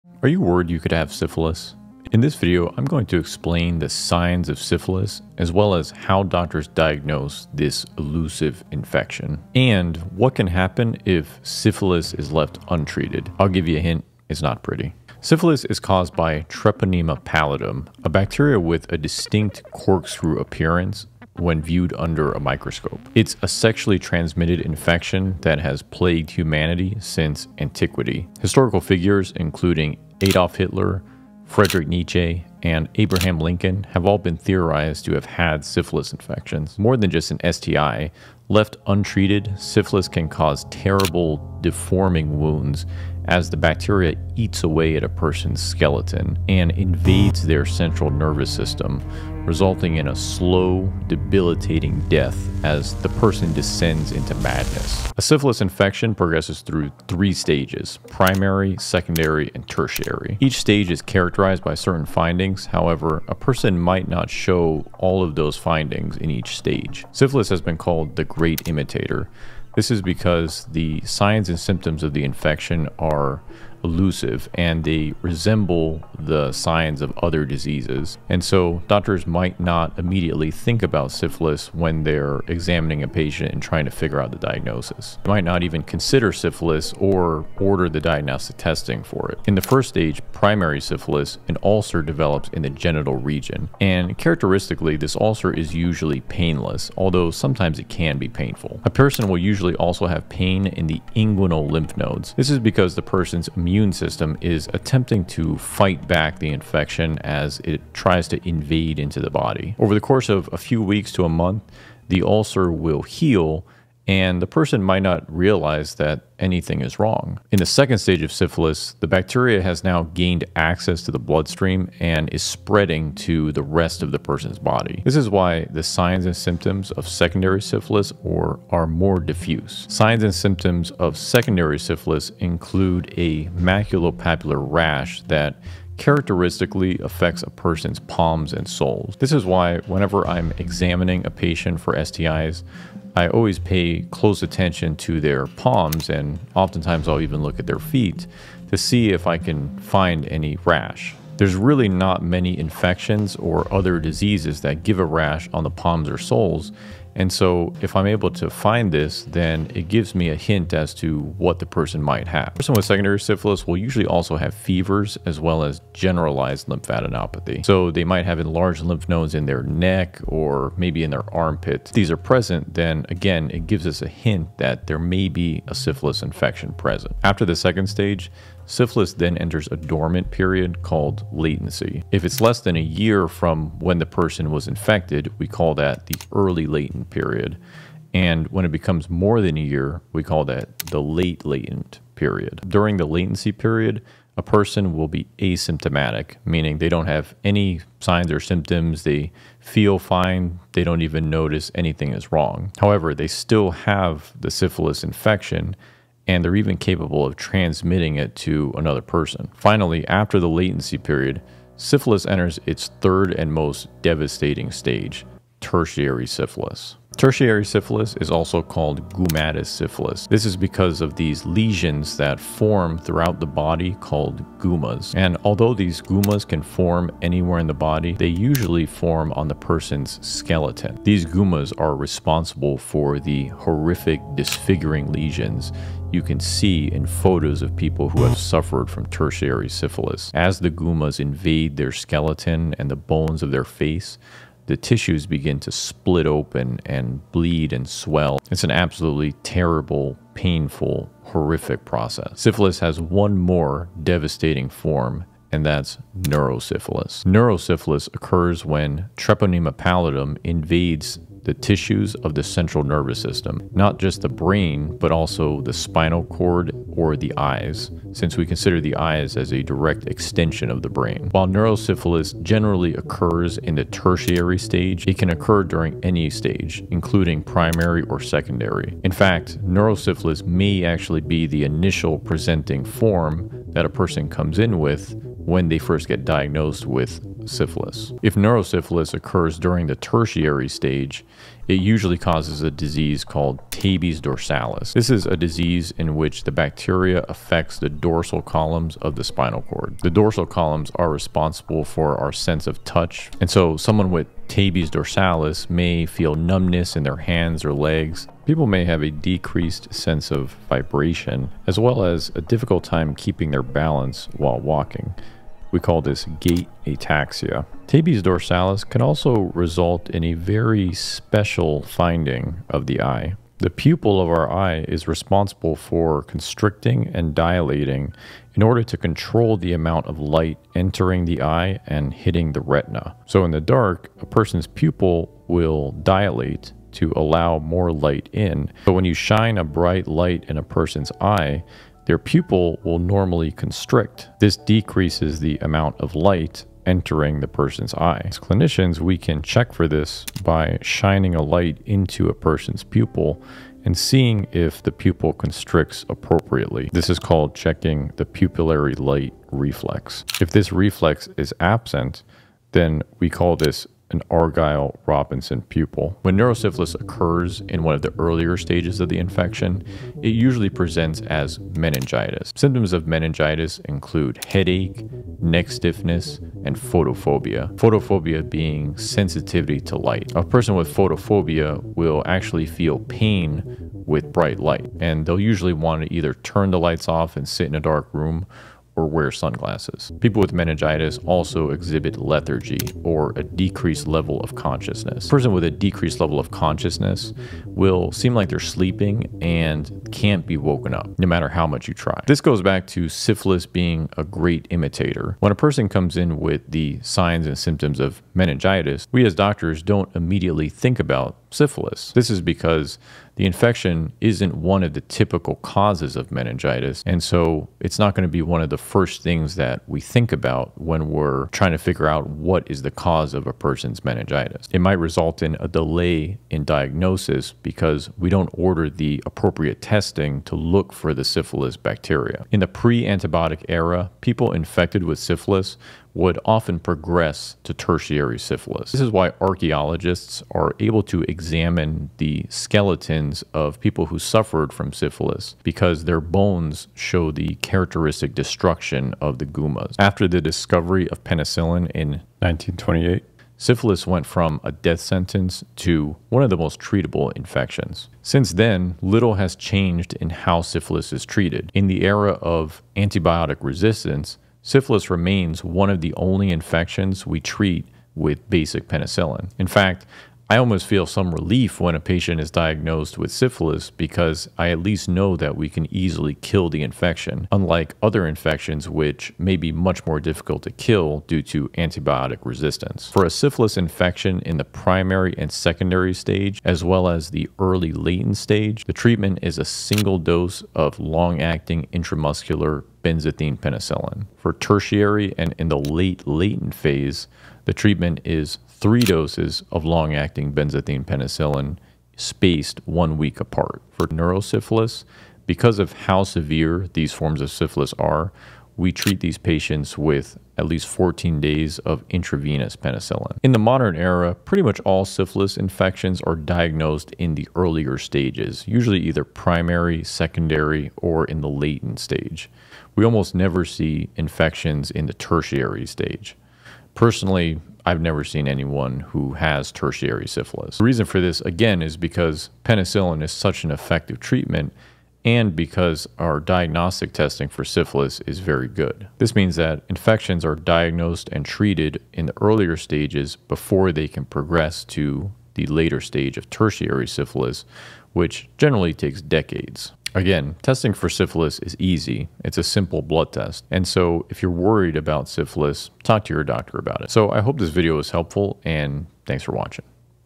The Three Horrifying Stages of Syphilis | Doctor Explains
This video discusses the stages, clinical findings, and treatment of syphilis from an infectious disease doctors perspective.

Chapters:
0:00 Introduction
01:30 Stage 1 Primary syphilis
01:58 Stage 2 Secondary syphilis
02:56 Latent syphilis
03:26 Stage 3 Tertiary Syphilis
04:12 Neurosyphilis
07:40 Treatment